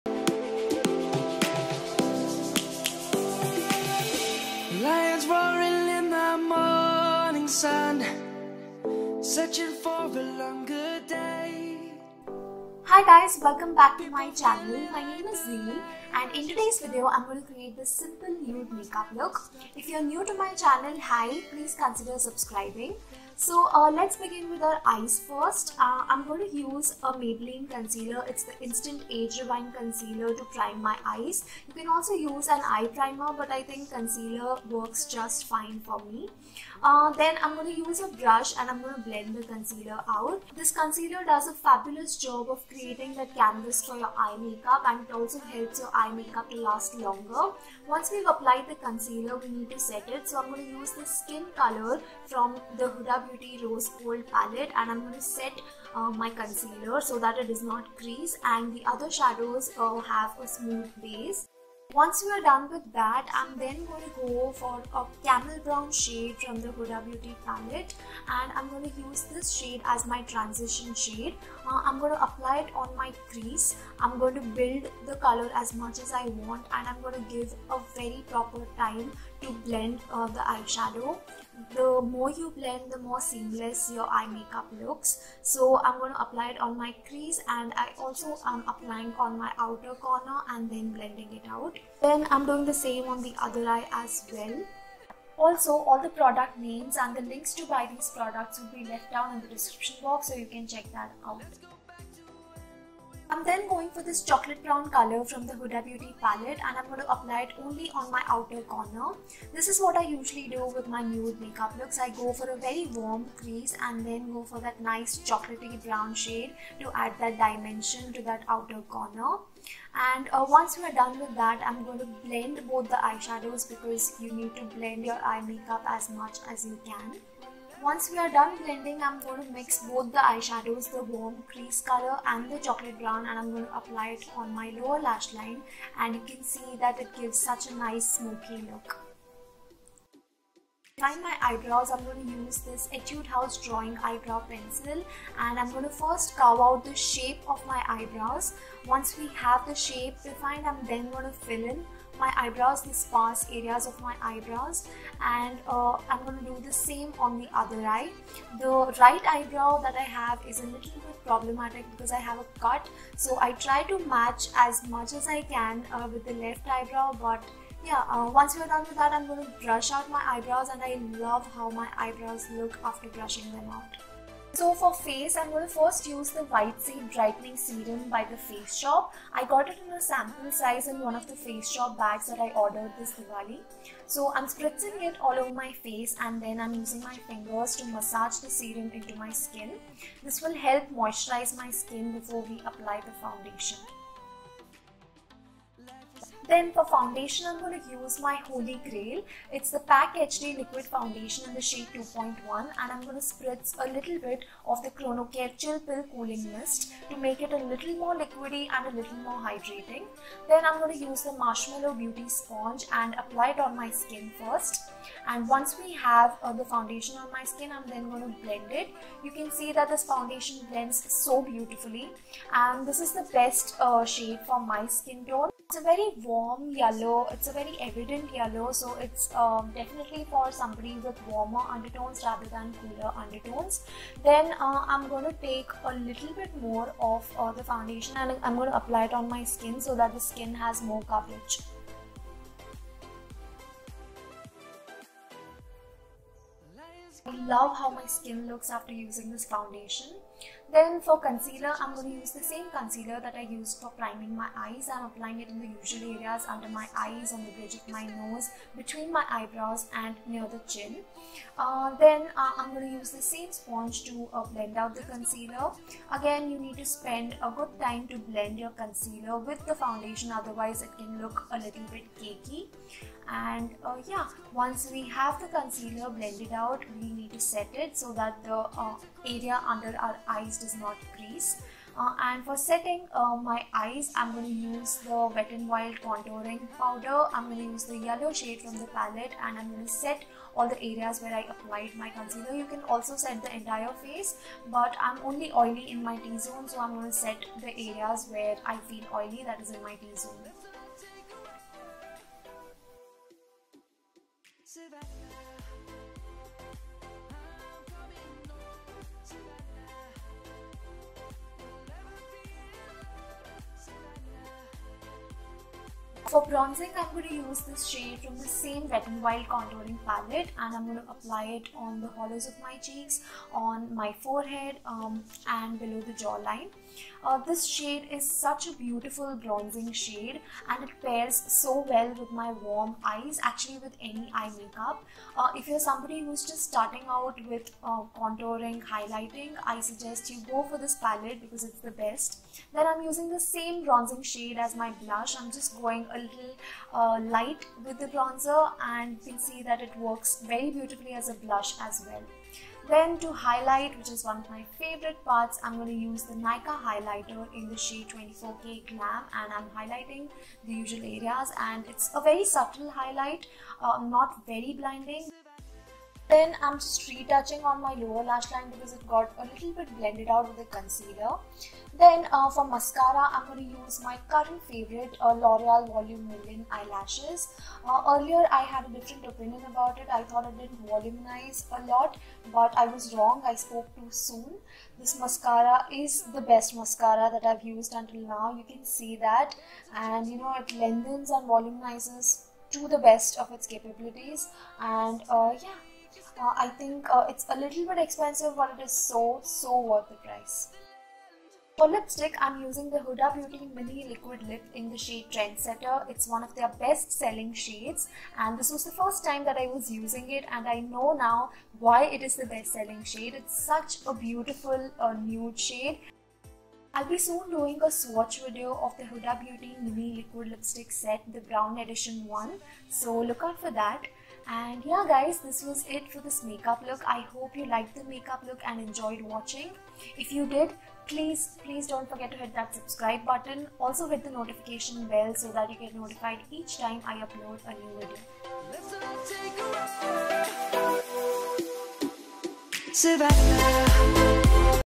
NUDE MAKEUP TUTORIAL FOR INDIAN SKIN | ZEAL THAKKER
NUDE MAKEUP FOR INDIAN SKIN

Hi guys,
I am creating a nude makeup tutorial for Indian skin in this video. I love warm eyes and nude lips so that's exactly what I'm doing in this video!